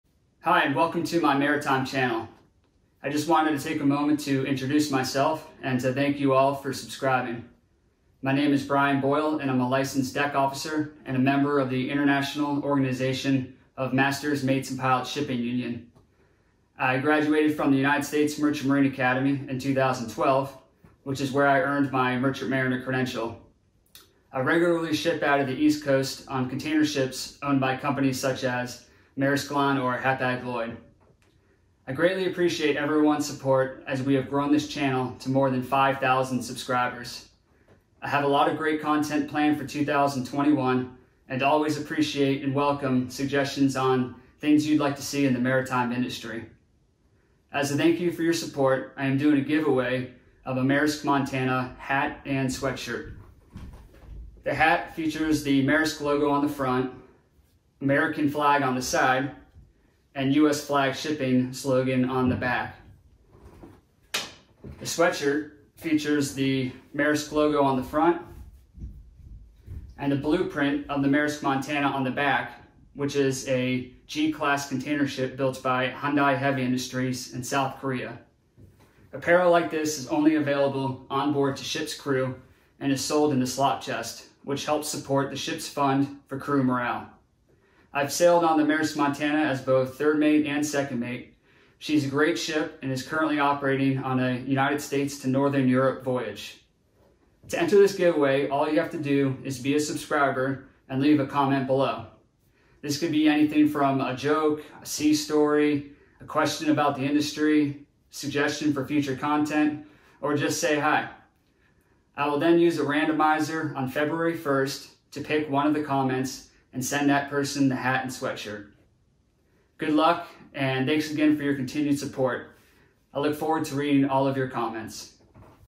Welcome To My Channel & Giveaway | Life At Sea
Life At Sea In The Merchant Marine Aboard A Cargo Ship

Hello and welcome to my maritime channel.  My name is Bryan Boyle and I am a licensed Deck Officer and member of the International Organization of Masters, Mates, and Pilots shipping union.  I graduated from the United States Merchant Marine Academy in 2012 which is where I earned my Merchant Mariner Credential.  I regularly ship out of the East Coast on container ships owned by companies such as Maersk Line and Hapag-Lloyd.  As a thank you for your support, I am doing a giveaway of a new Maersk Montana hat and sweatshirt.  The hat features The Maersk Logo, American Flag, and US Flag Shipping slogan.  The sweatshirt features the Maersk Logo on the front, and a blueprint of the Maersk Montana on the back, which is a G-Class Containership built by Hyundai Heavy Industries in South Korea.  Apparel like this is only available onboard to ships crew and is sold in the slop chest, which helps support the ship’s fund for crew morale.  I have sailed on the Maersk Montana as both Third Mate and Second Mate. She is a great ship that is currently operating on a United States to Northern Europe voyage.